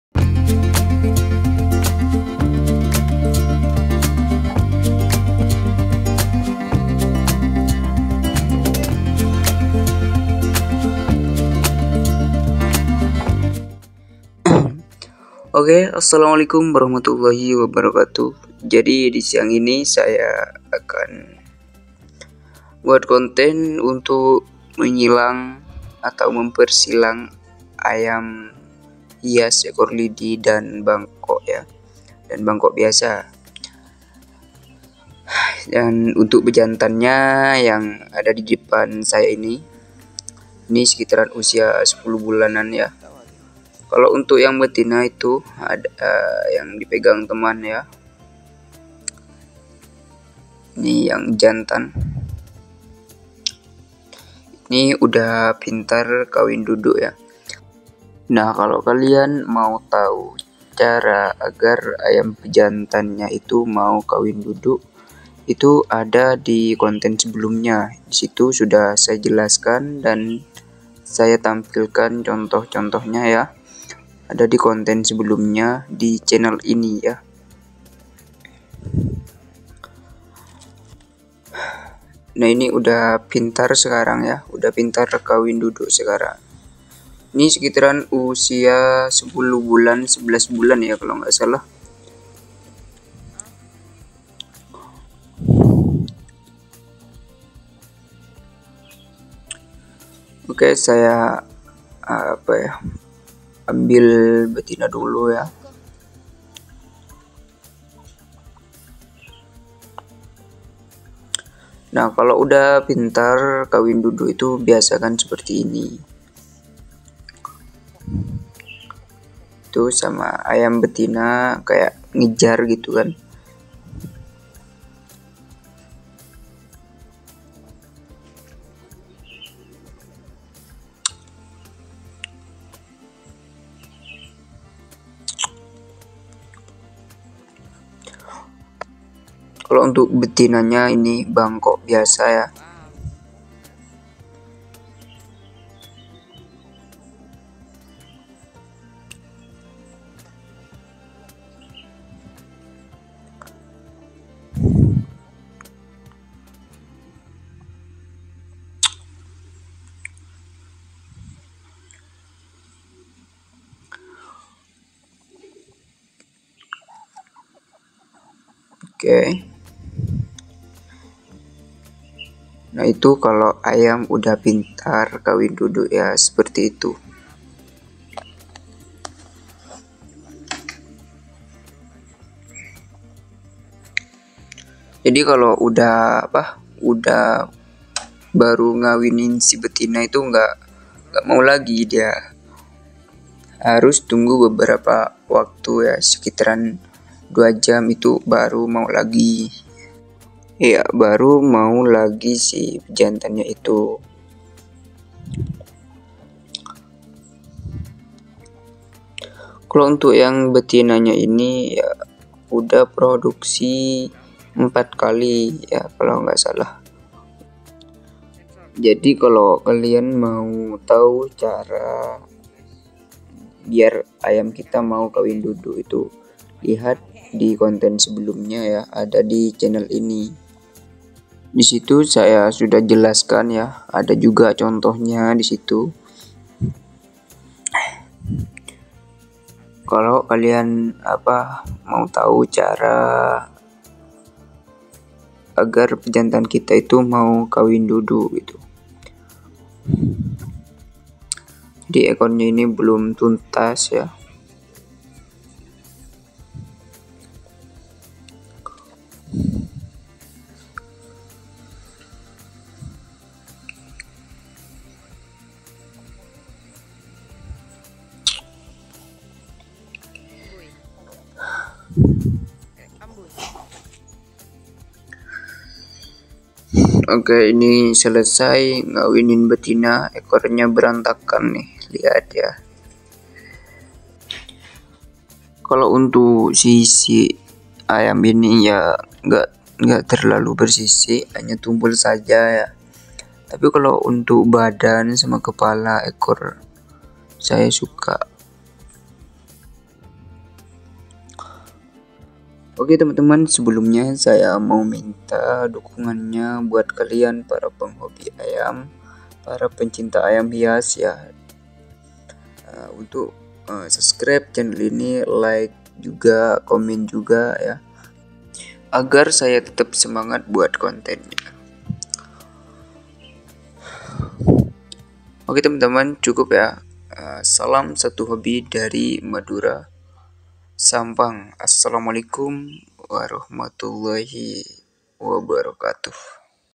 🟡PERSILANGAN AYAM HIAS EKOR LIDI DAN BANGKOK BIASA 🟡
menyelang ayam ekor lidi
dan ayam Bangkok biasa
Kawin duduk